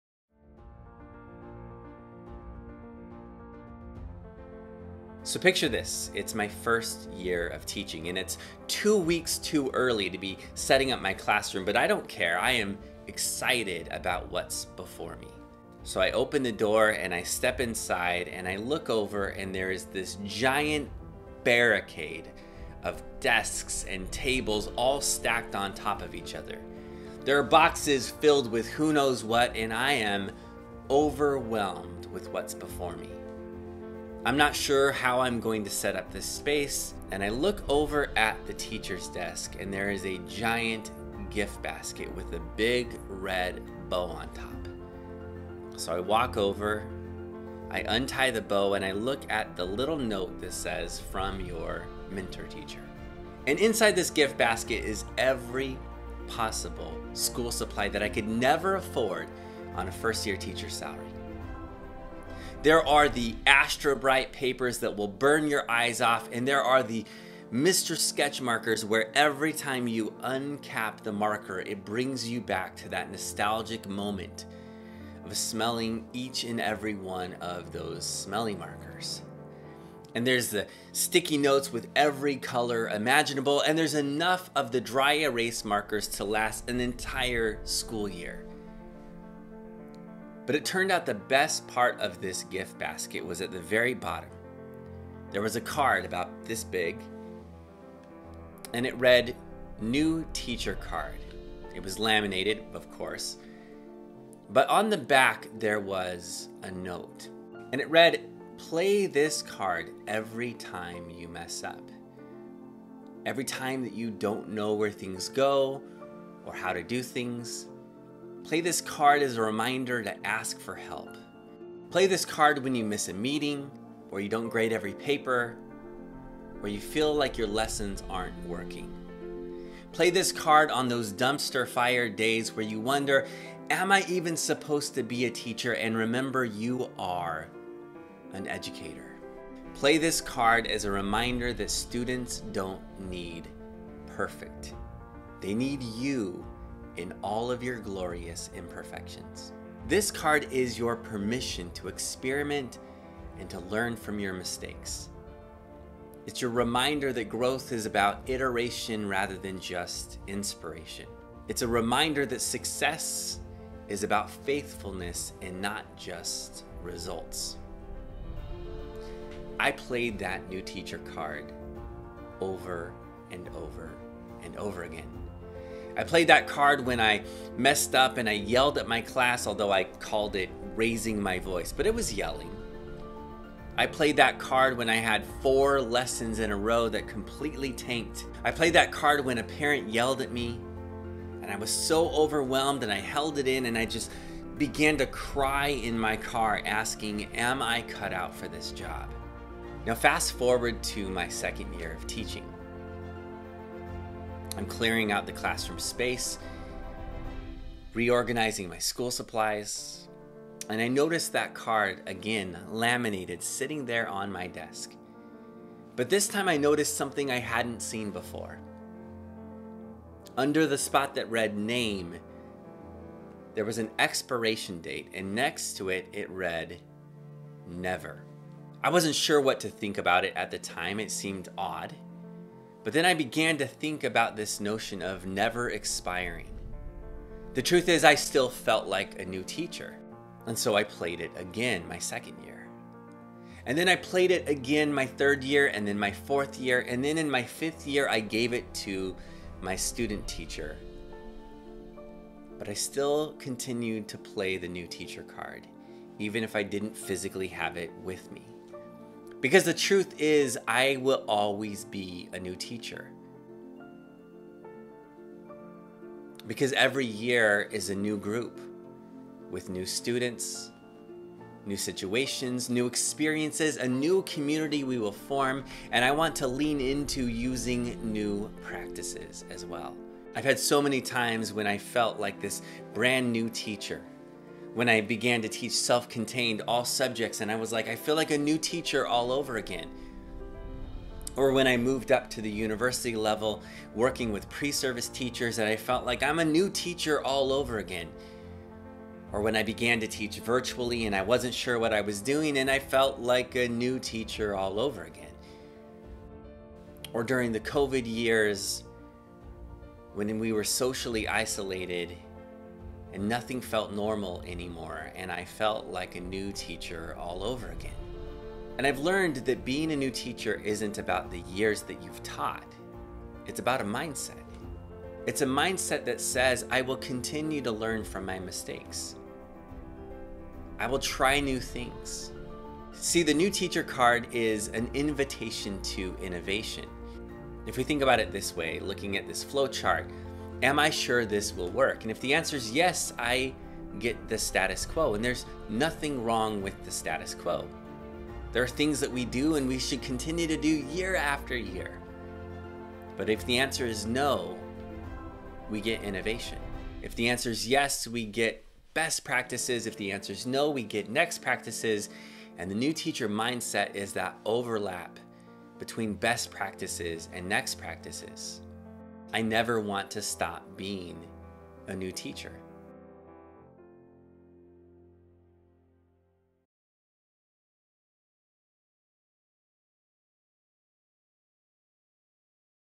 Always Play the New Teacher Card
🌟 My Journey as a New Teacher | Embracing the New Teacher Mindset 🌟

In this video, I take you through the roller coaster of my first year of teaching—the excitement, the challenges, and the overwhelming moments. From stepping into my classroom for the first time to discovering an unexpected gift from my mentor teacher, I learned valuable lessons that have shaped my teaching philosophy.

🎁 The New Teacher Card: A simple yet powerful reminder that being a teacher isn’t about perfection—it's about growth, experimentation, and embracing the glorious imperfections that make us who we are. Whether it’s navigating a chaotic classroom or dealing with the tough days, this mindset has stayed with me through every year of my teaching journey.

✨ Why the New Teacher Mindset Matters: Every year brings a new set of students, experiences, and challenges, and with them, the opportunity to innovate and grow. In this video, I share how this mindset has transformed my approach to teaching, from elementary school to the university level, and why I believe it’s crucial for educators at every stage of their careers.

🔗 Join me as I reflect on my journey and explore how we can all embrace the New Teacher Mindset to foster creativity, resilience, and continuous learning in our classrooms.